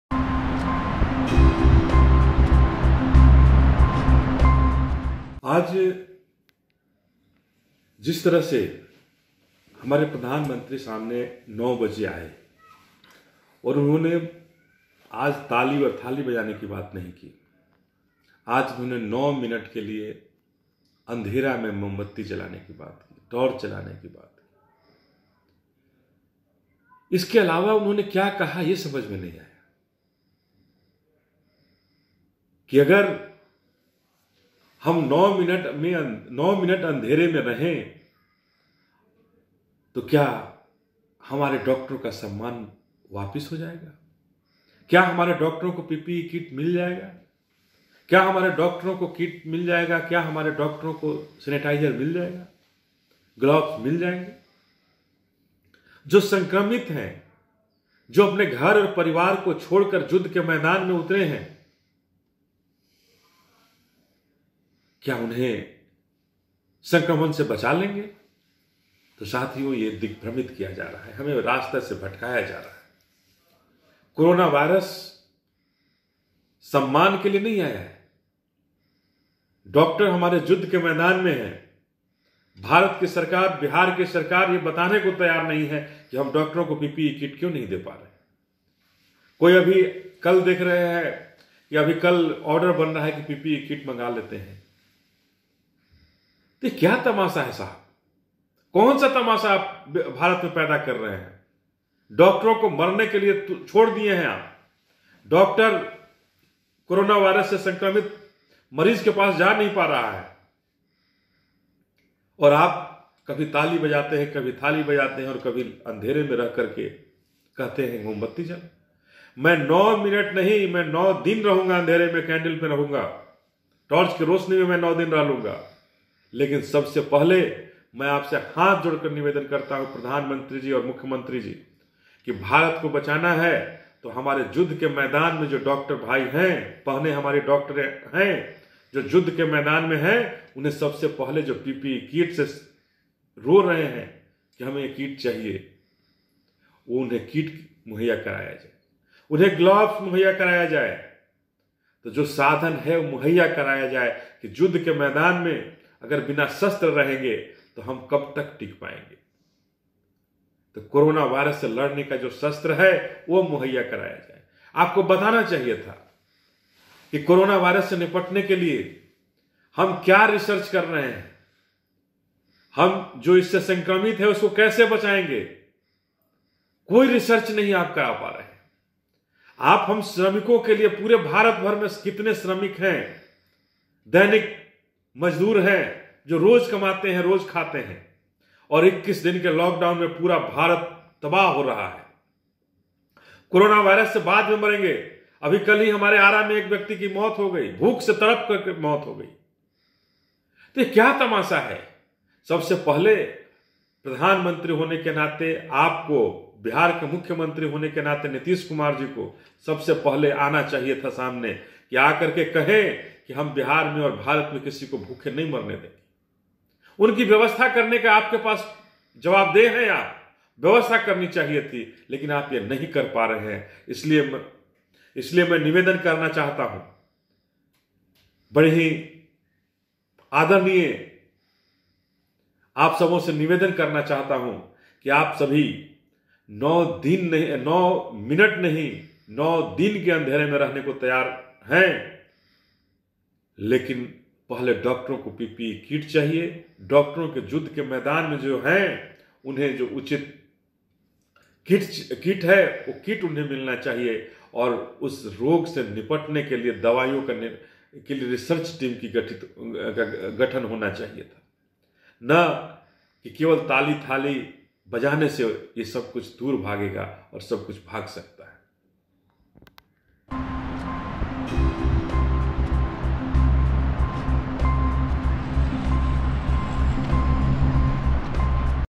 PM Modi के संबोधन पर जमकर बरसे JVP अध्‍यक्ष Anil Kumar, कहा – बंद करिये ताली – थाली की नौटंकी
PM Modi के संबोधन पर जमकर बरसे JVP अध्‍यक्ष Anil Kumar, कहा – बंद करिये ताली – थाली की नौटंकी | LiveCities